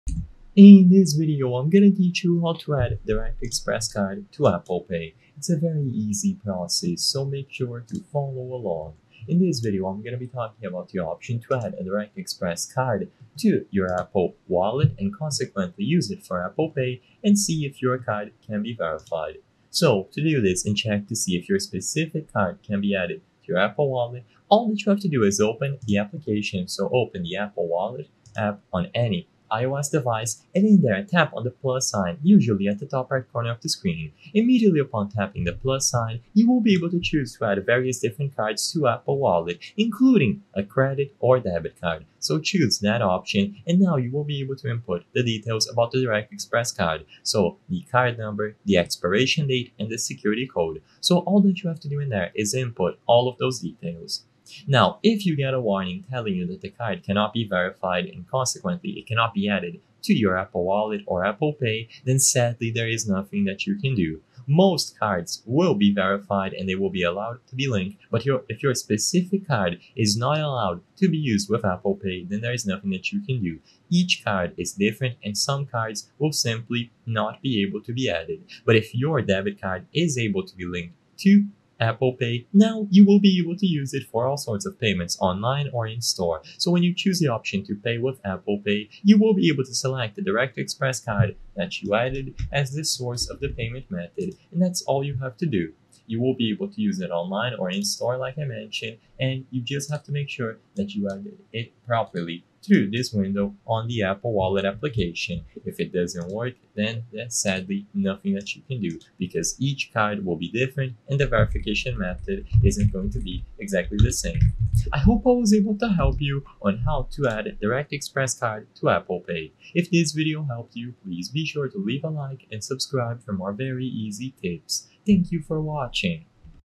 How To Add Direct Express Card to Apple Pay 2025 - Step by Step
In this video I will clear your doubts about how to add direct express card to apple pay, and whether or not it is possible to do this.